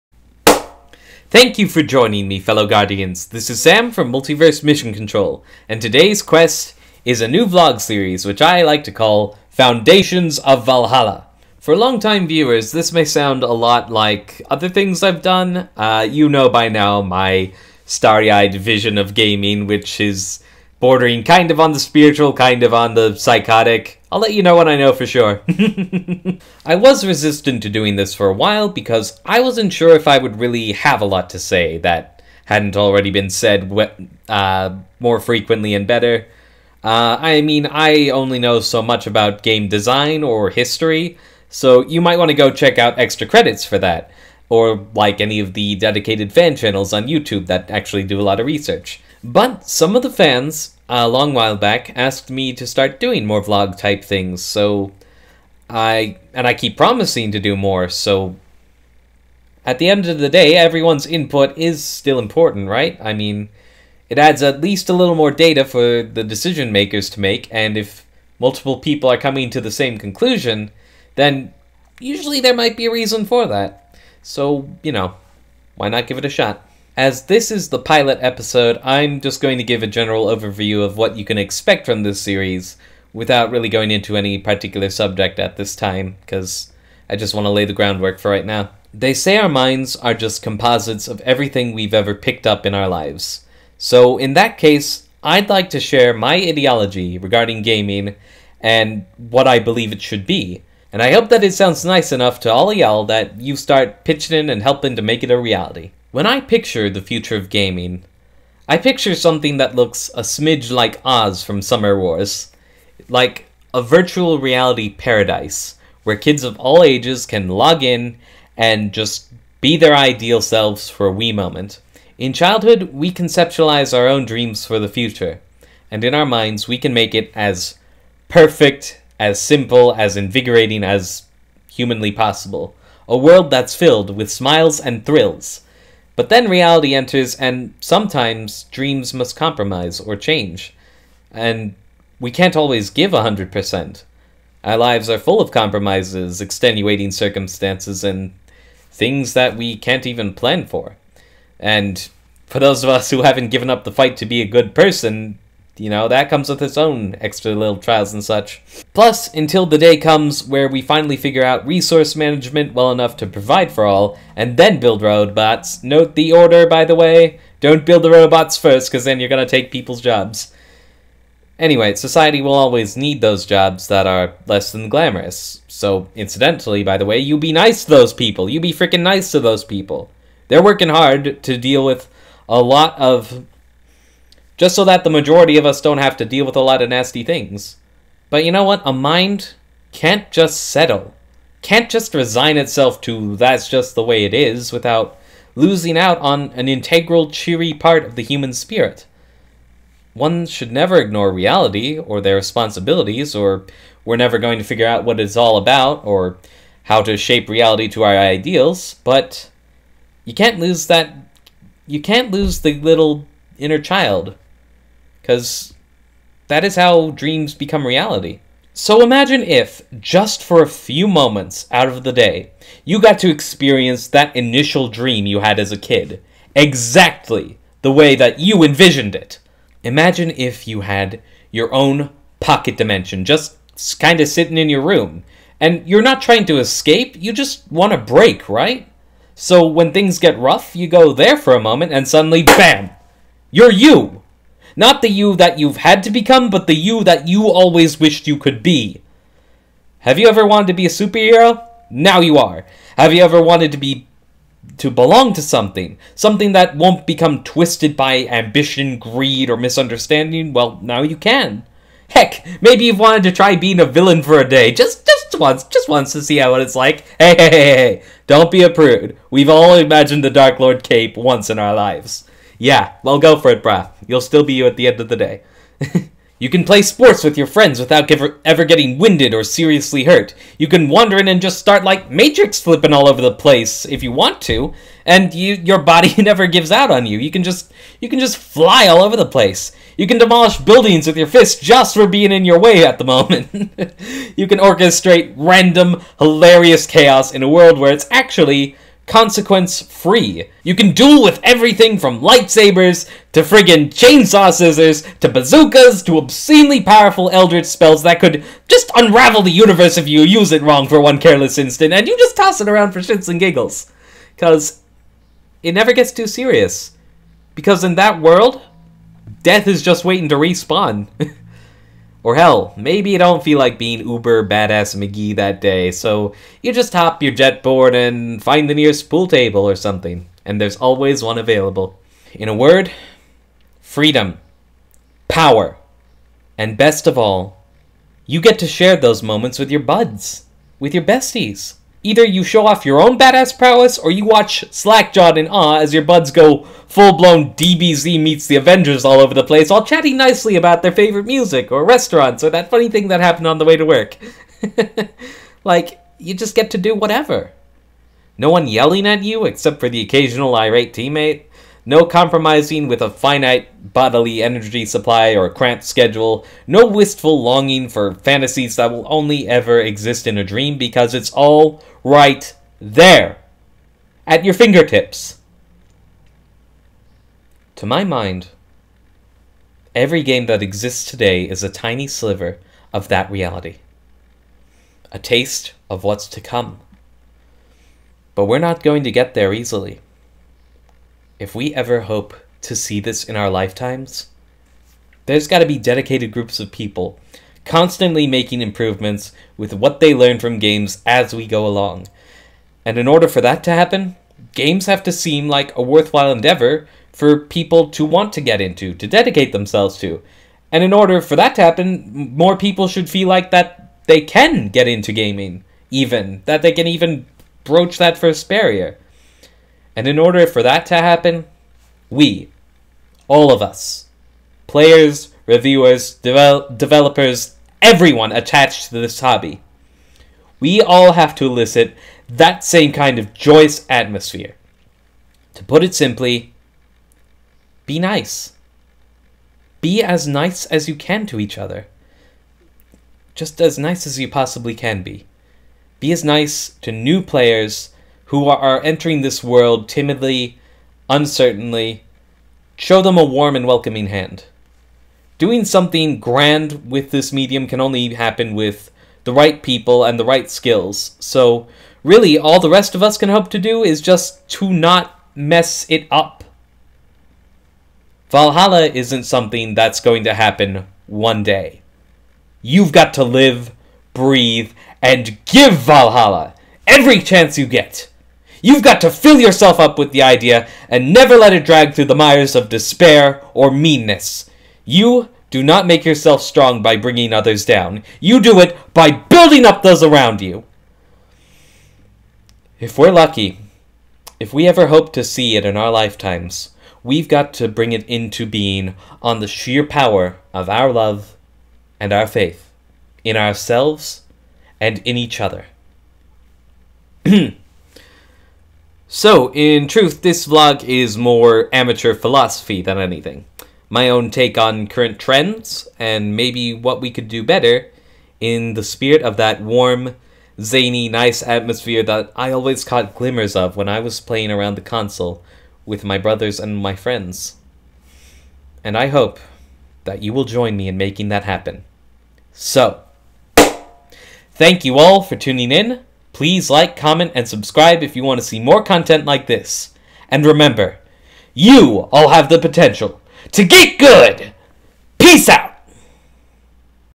Foundations of Valhalla Episode 1 | A Vision of the Future - Multiverse Mission Control
[If the video stops buffering, click on the red bubble in the loading bar. The video works fine, you just have to show it who's boss first, wot?]

In this episode, I briefly describe the future of gaming as I saw it in my childhood and the most simple rule everyone can do to accomplish it.